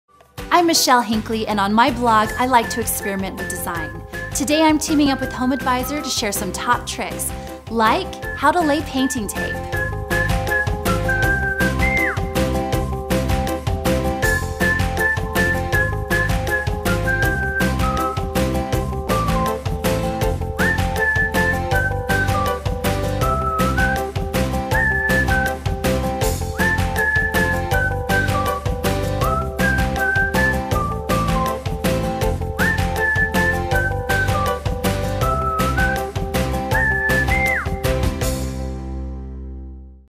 How to Lay Painting Tape | Home Hack
Blogger Michelle Hinckley of 4men1lady.com shows you five easy ways to lay interior painting tape for a perfect line between walls and trim every time.

Visit homeadvisor.com to hire a pre-screened home service pro to complete this task.